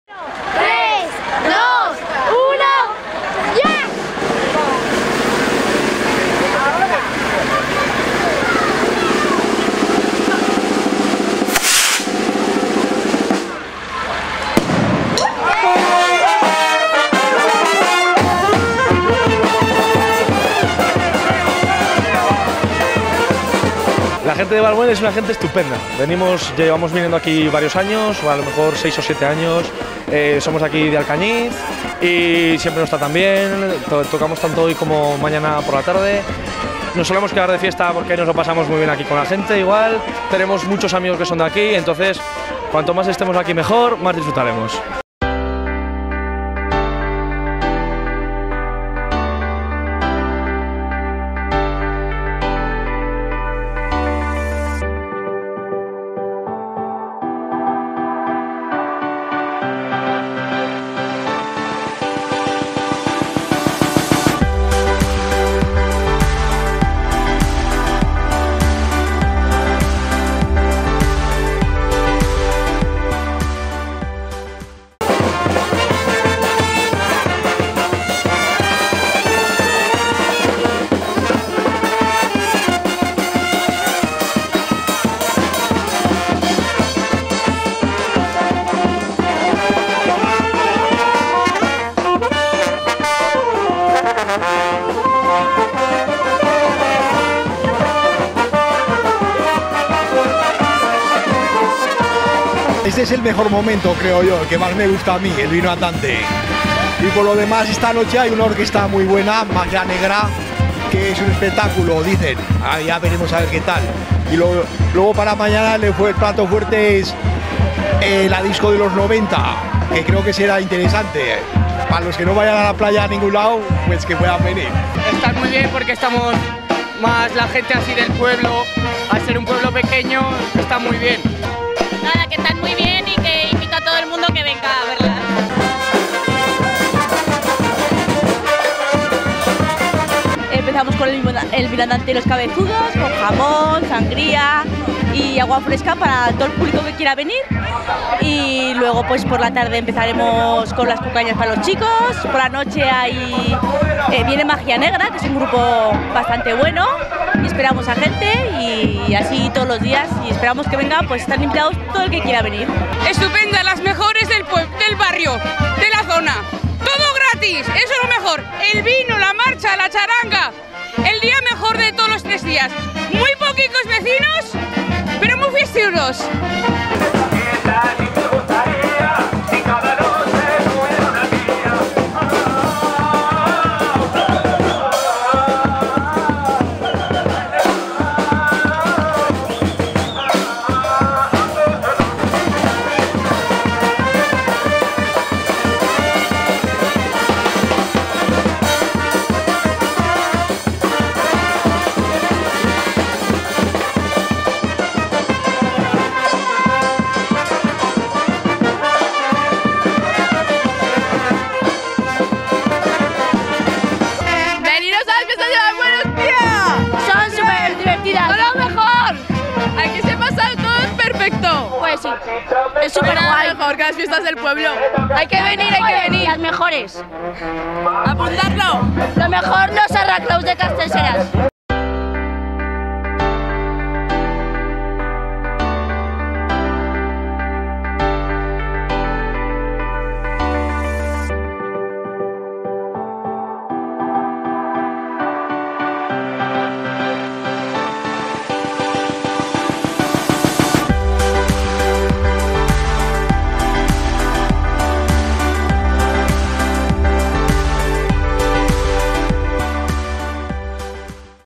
Vino andante para empezar las fiestas de Valmuel
Las fiestas de Valmuel ya están aquí. Tras el cohete de inicio la charanga "Los Sonaos" han recorrido las calles mientras la comisión ha repartido sangría y jamón serrano a todo el mundo. Mucha animación que continuará hasta el próximo domingo por la noche. No faltes a Valmuel.